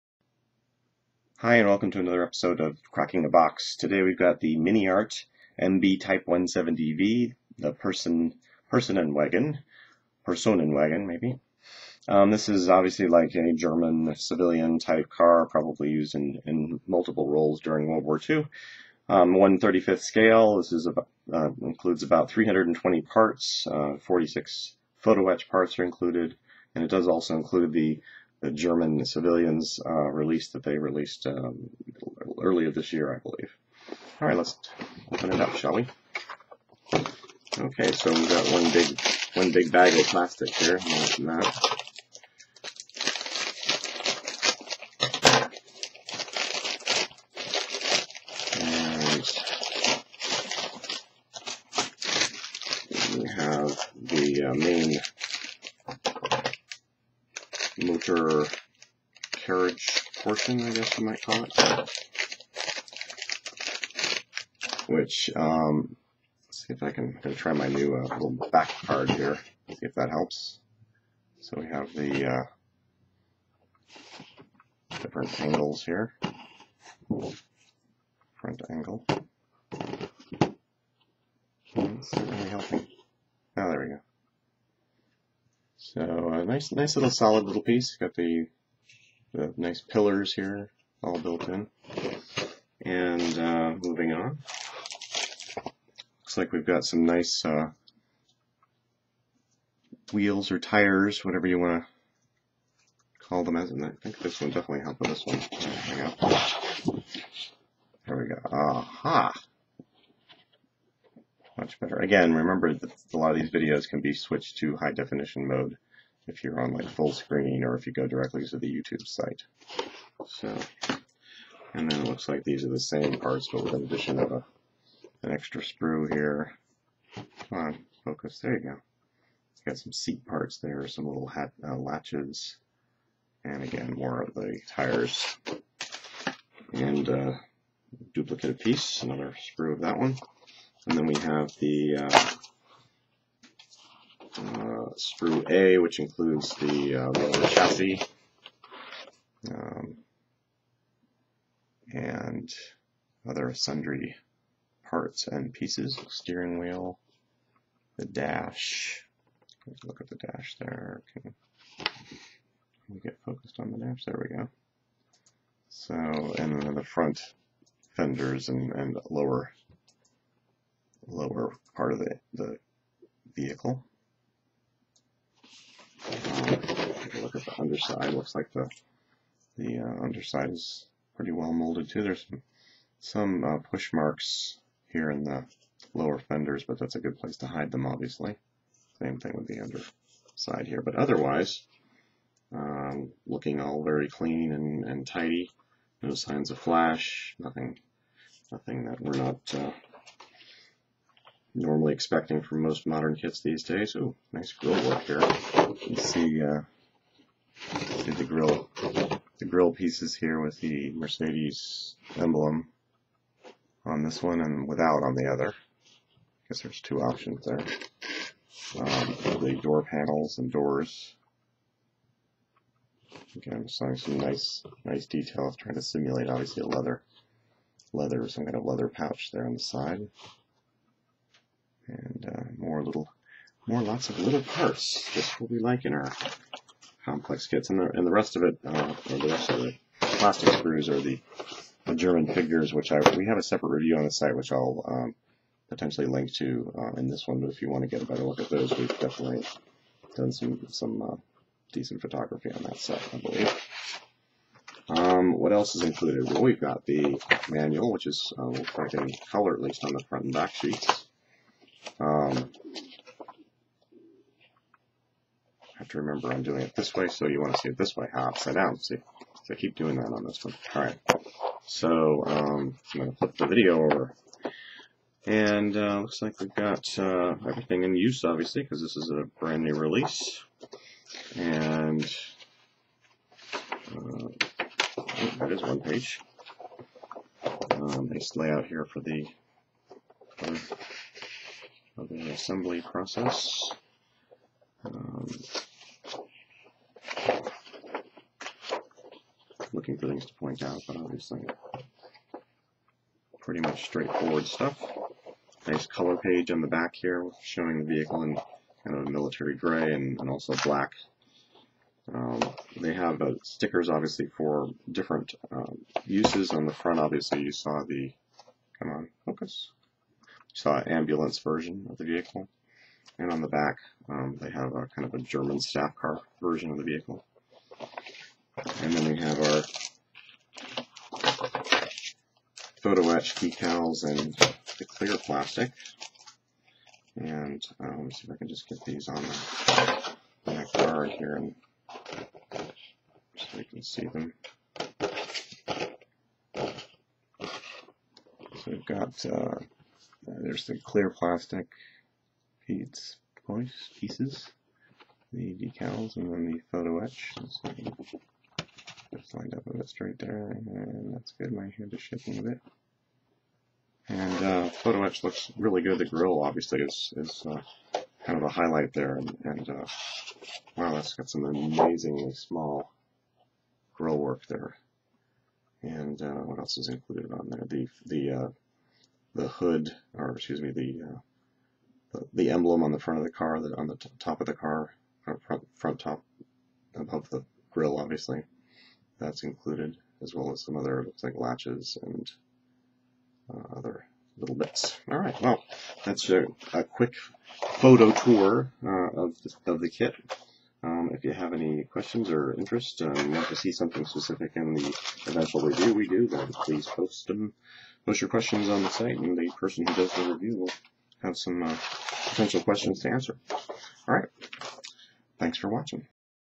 MiniArt MB Type 170V Personenwagen (35095)
We take a quick look at this new kit.